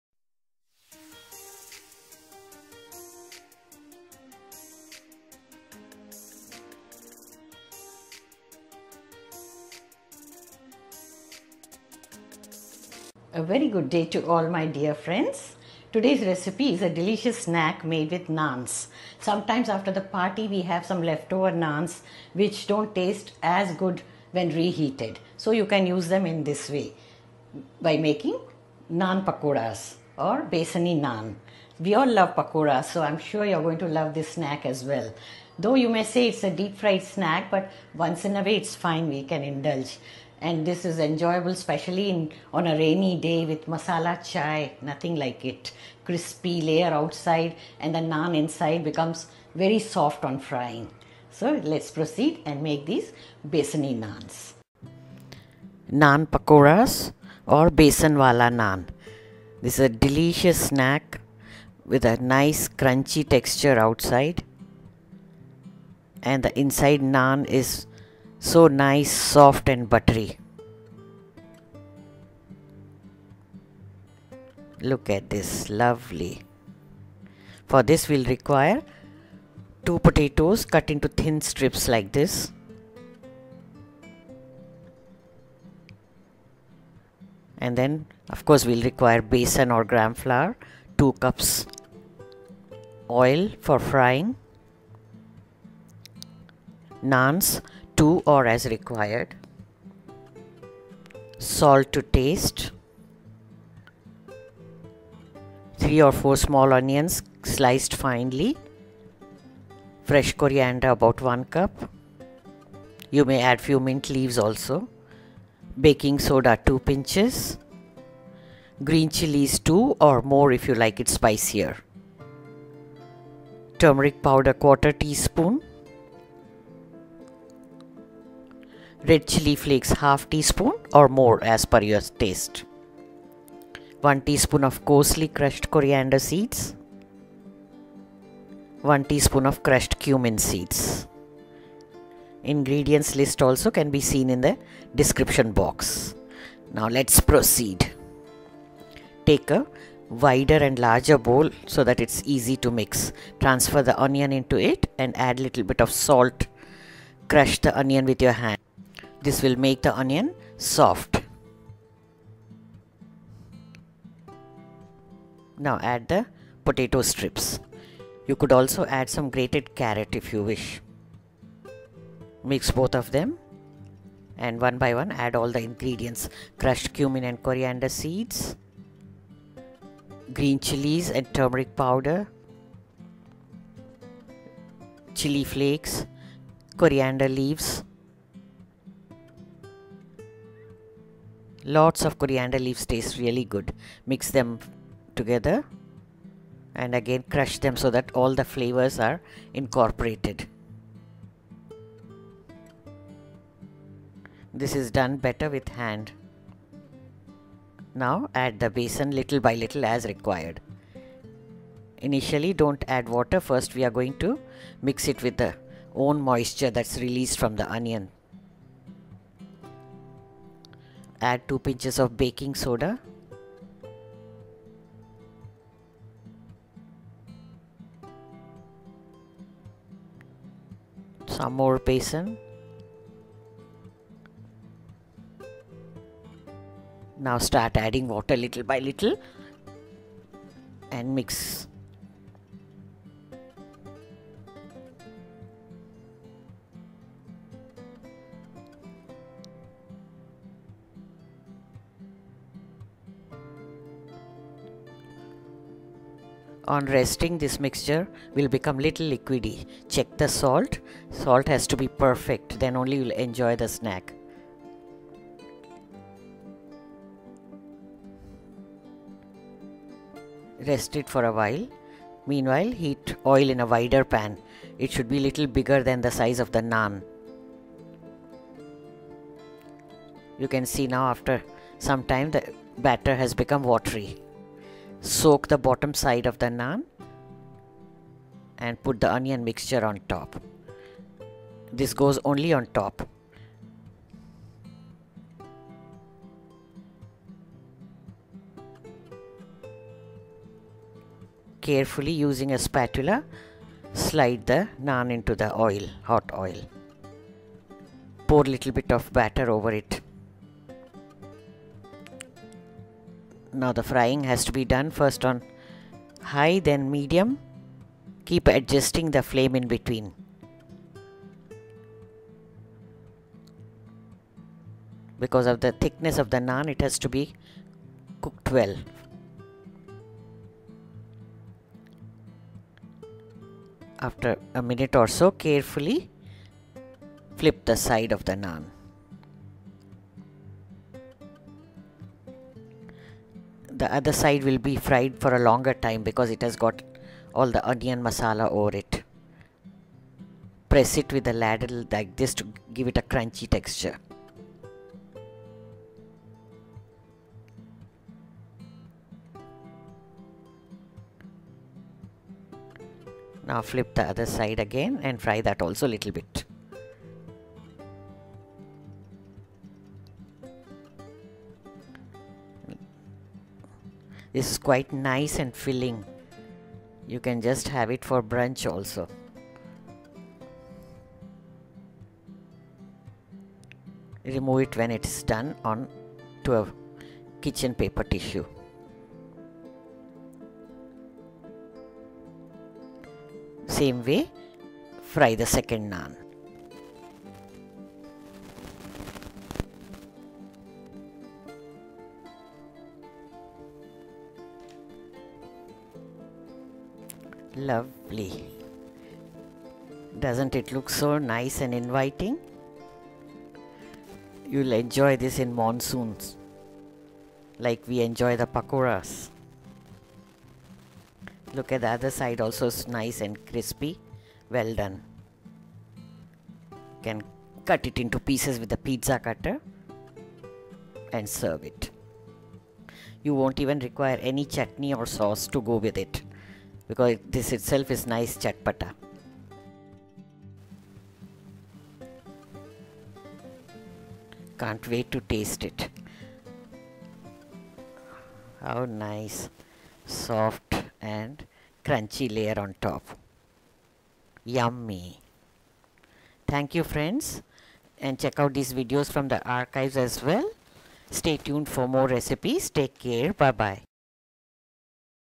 Naan Pakoras | Besan Wala Naan | बेसन वाले नान | नान के पकौड़े
If you have any leftover naans after a party try making this !

INGREDIENTS:
Naans - 2 or as needed
Besan / gram flour - about 2 cups
Onions - 3 or 4
Potatoes - 2 medium
Coriander leaves - 1 cup
Green chillies - 2 or 3
Baking soda - 2 pinches
Red chilli flakes - 1/2 tsp or more
Coarsely crushed coriander seeds - 1 tsp
Crushed cumin seeds - 1 tsp